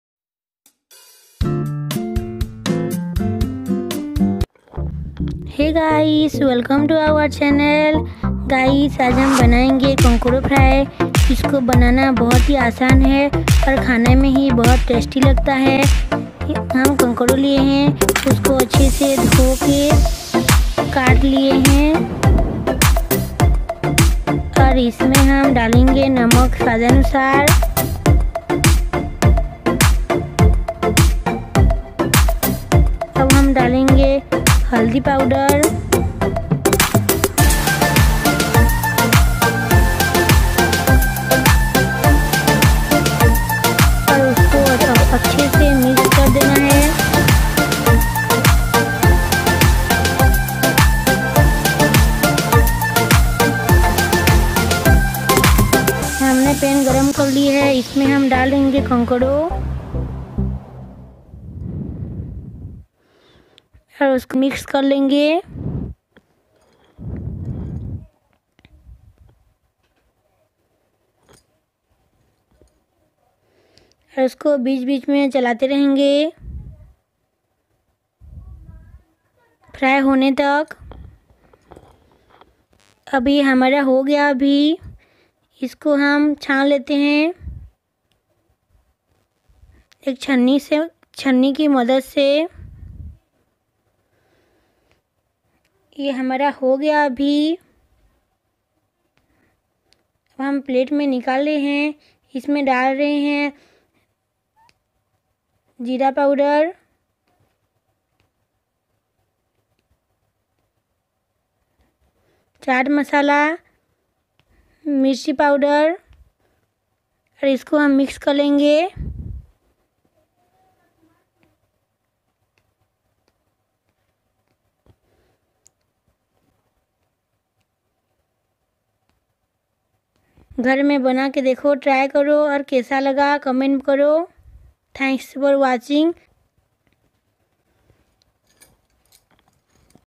Easy & Quick Recipe!! Testy !Konkado(konkorlo) Fry.. How To Make..Kitchen With Fun Shibani Ki Arya.
easy&quickrecipe kitchenwithfunshibanikiarya testy konkadofry.

INGRIDIENTS :-
100gm konkado (konkorlo).
1/4tps haldi powder.
salt to taste..
some oil for fry.
chat masala...
jeera powder ...(roasted)
black salt...
chili powder..

kitchen with fun Shibani ki arya ...